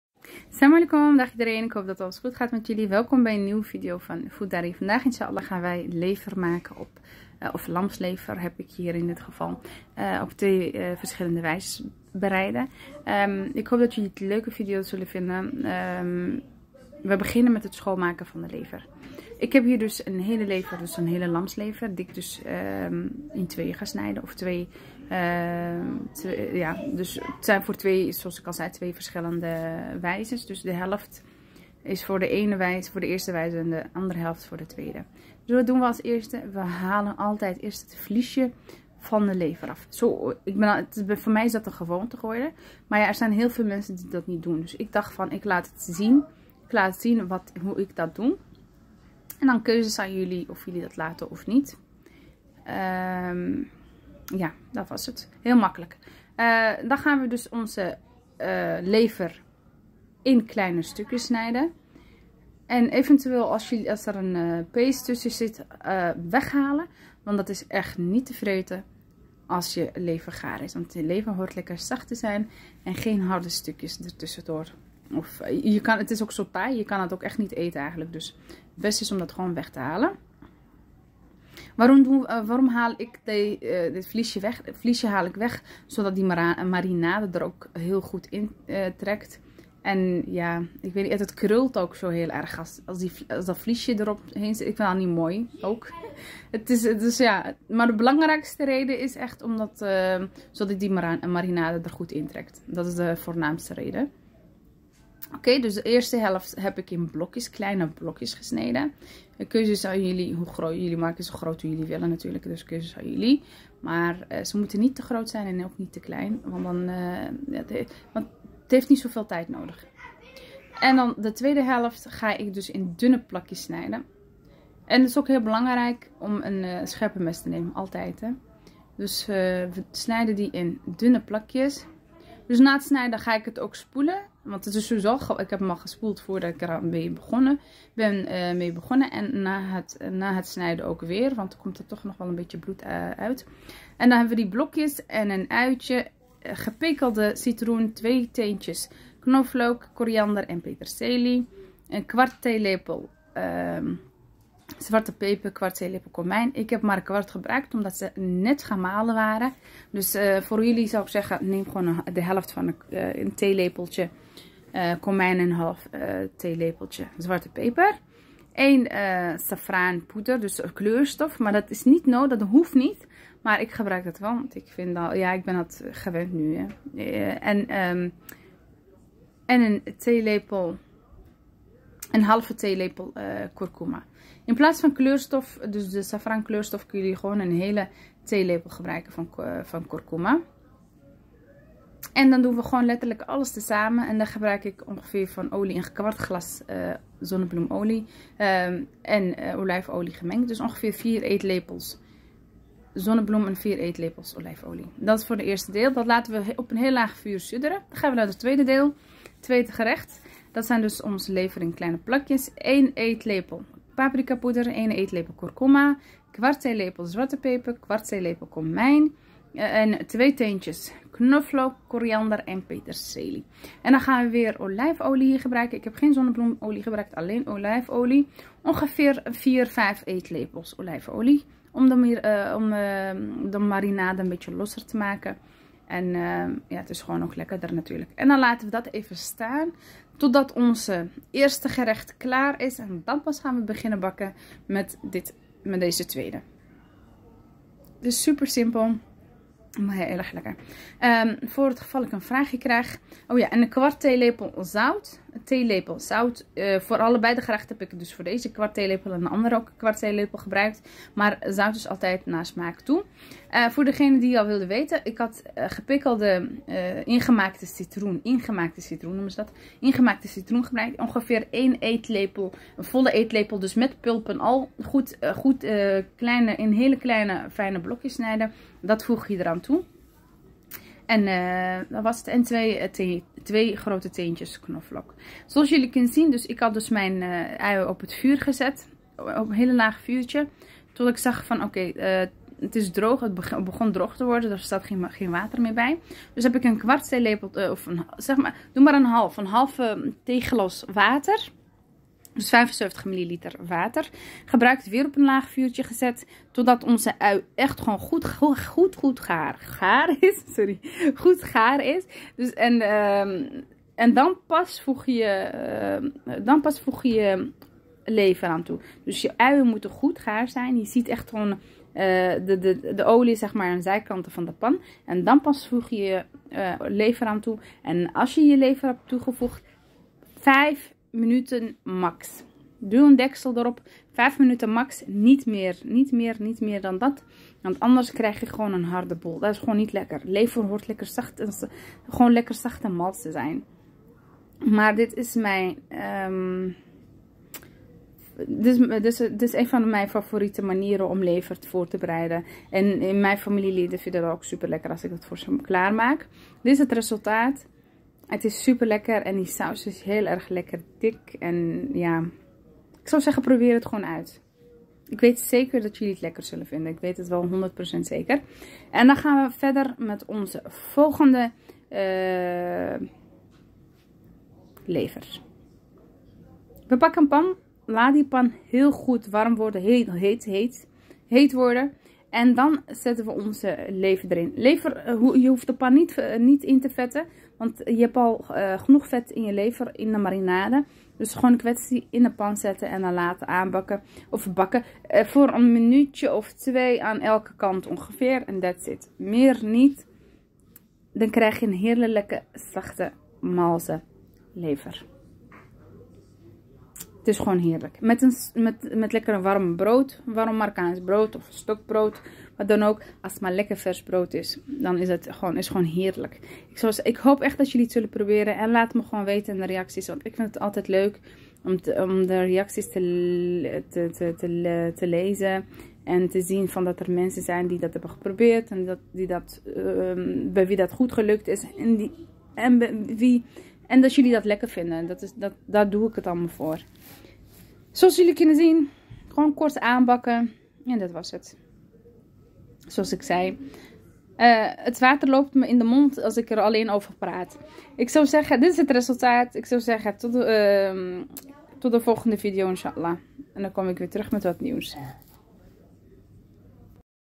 Lamslever op Marokkaanse manier 2 verschillende bereidingen super lekker en makkelijk.
Voor meer gezelligheid en recepten klik op de link hieronder en voeg je toe aan mijn kookgroep.